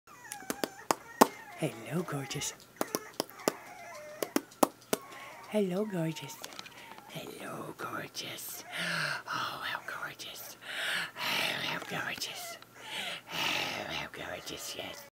Video of adoptable pet named Sonya
Sonya is an adoptable pet with Richardson Rescue in York SC.